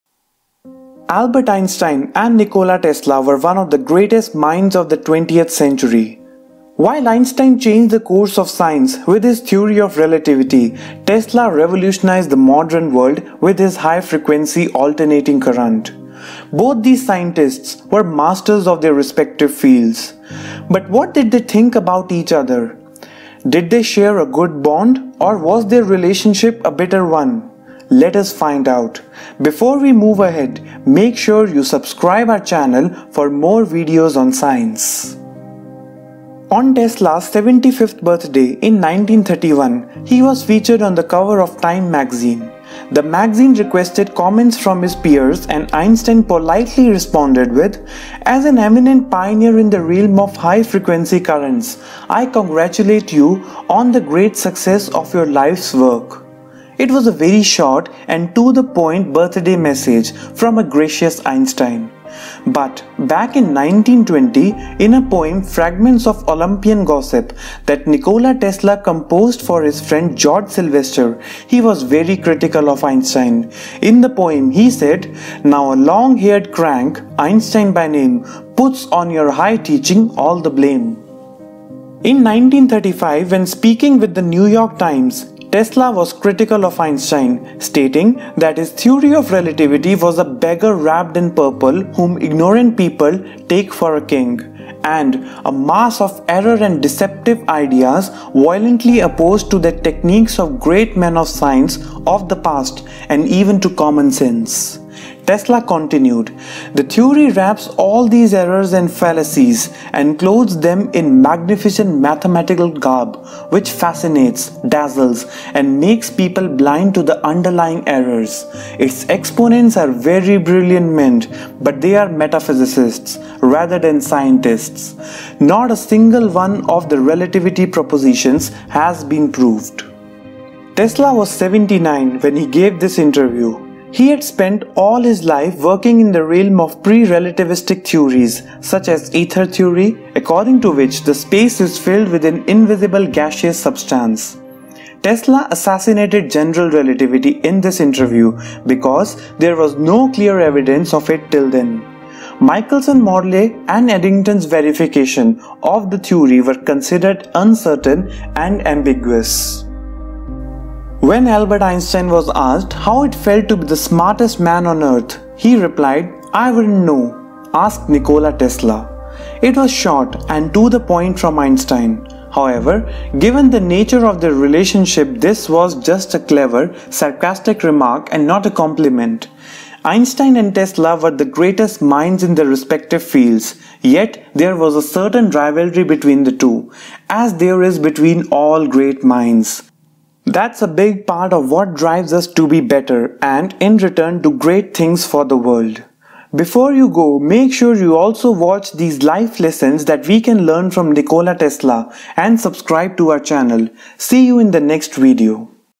Tesla And Einstein Hated Each Other And Here Is What They Said!
Tesla and Einstein were two of the greatest minds in the history. They both played an integral role in their respective fields. While Einstein developed the theory of relativity and changed the laws of physics, Tesla was the architect of the modern world with his innovative developments such as alternating current. But what did the two think of each other? Why Einstein and Tesla hated each other and what did they say?